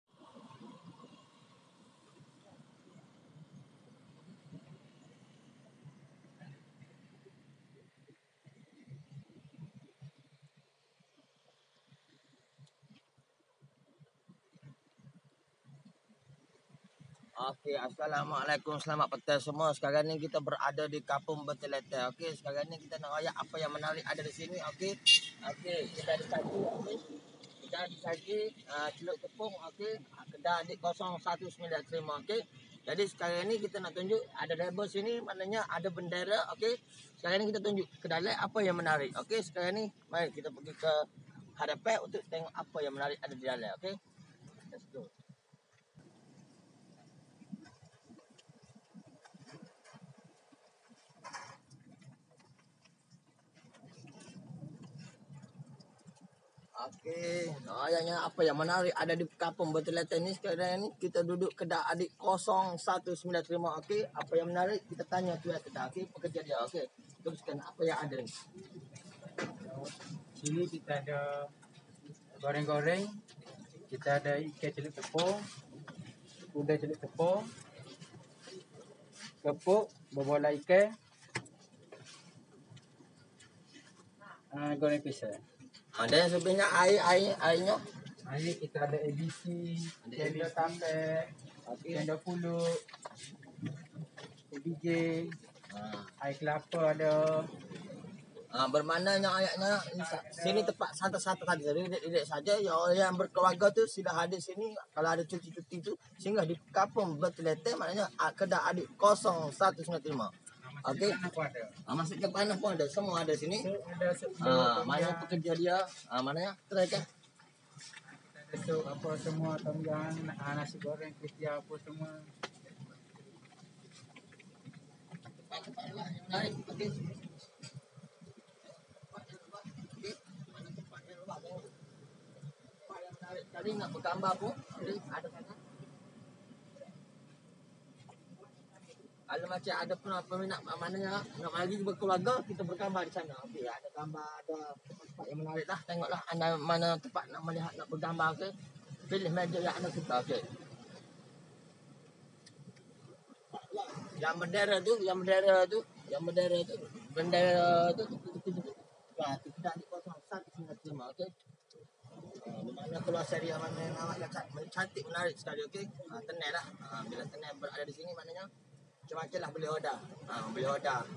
Jalan Jalan Cari Makan Malaysia |D' saji Celup tepung, di pantai kampung Benting Lintang, Besut.
Video from Malaysia The Rock Channel 786 Ususniaga: D'saji Celup tepung, Buka
mula Jam 11 pagi sampai jam 7 petang.

Buka setiap hari, Di Pantai Kampung Benting Lintang, Besut, Terengganu.

Alhamdulillah.. Terima kasih..

Harap maaf sangat2 kerana video ini kurang jelas kerana wayerless microphone kami tiba2 rosak..